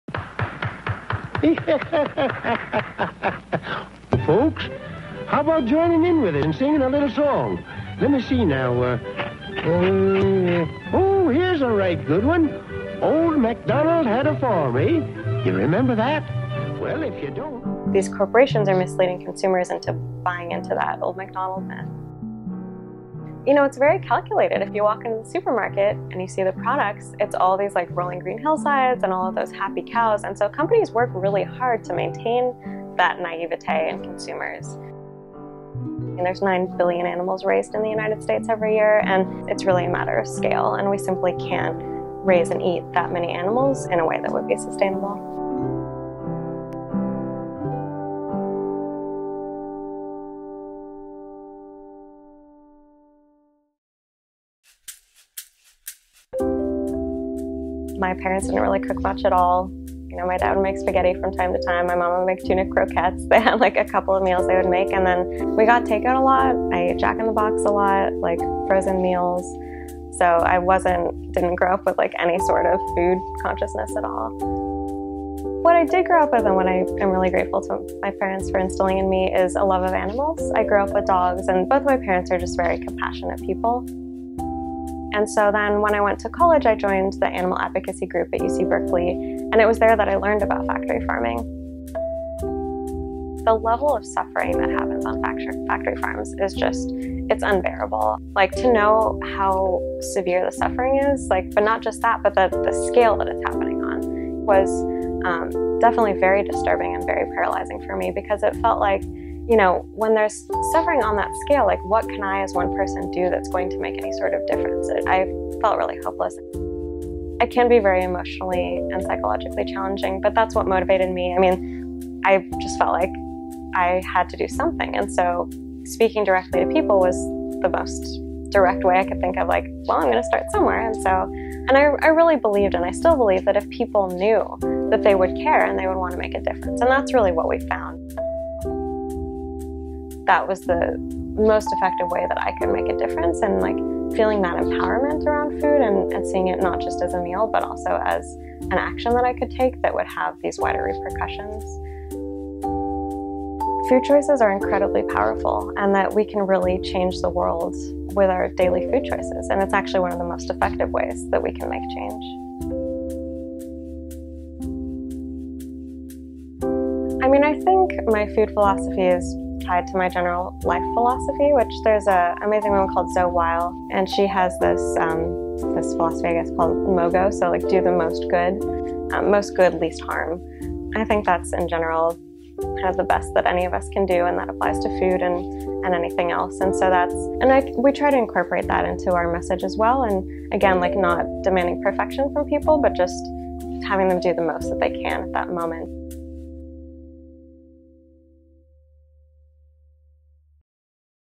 Eat Aware: The Hidden Costs of Meat
Meet Katie Cantrell, director of the Factory Farming Awareness Coalition. Katie tackles sustainable eating by addressing one major concern: our food choices. What if we ate less meat and move towards a plant-based diet?

Video Credits to CompassionUSA & Livestock & Poultry Environmental Learning Center